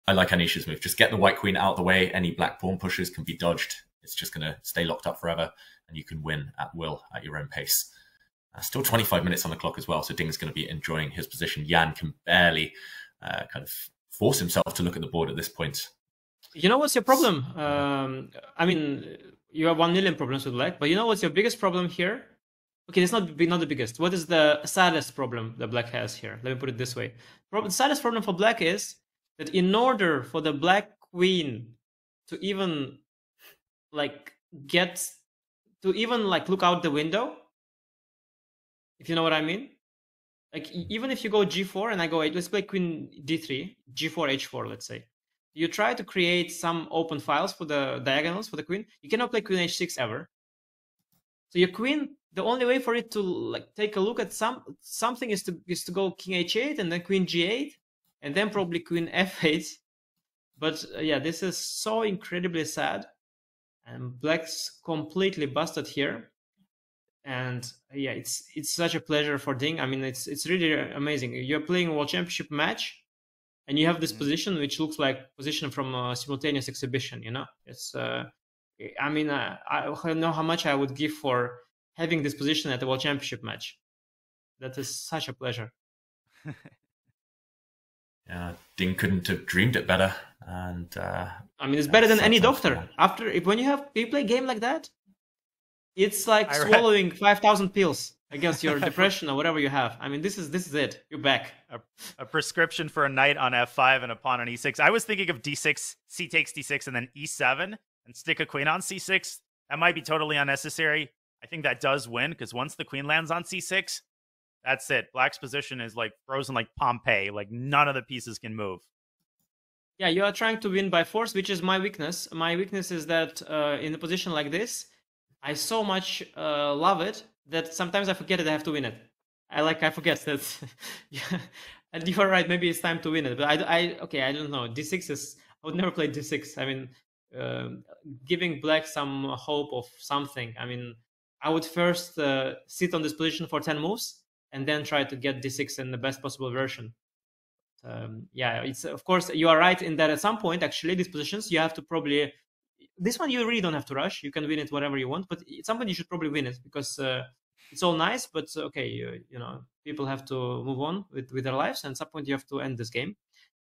"I'd sit on this position for 10 more moves" - Anish Giri
Anish Giri on totally winning position of Ding Liren in World Chess Championship, Ian Nepo vs Ding Liren Game 4, talks about the beauty of the position for Ding Liren.